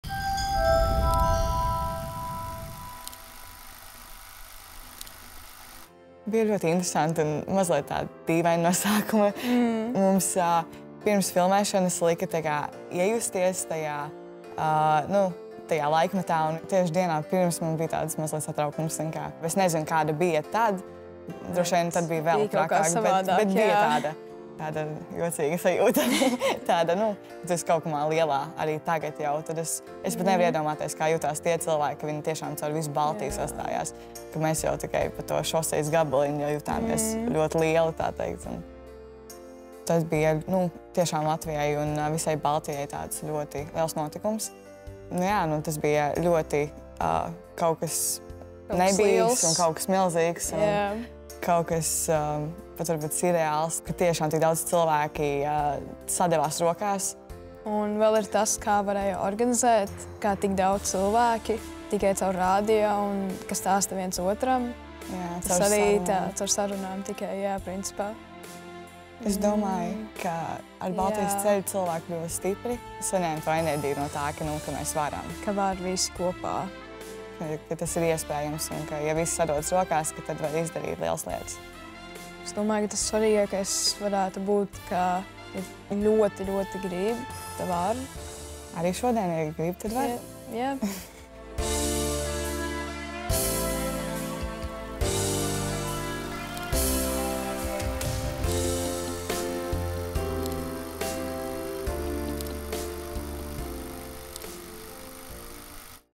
Baltijas ceļš 30
Baltijas ceļš ir notikums ne vien tiem, kuri tajā piedalījās, bet arī tiem, kuri tajā laikā vēl nebija dzimuši, jo tas pavēra ceļu Latvijas un visas Baltijas brīvībai.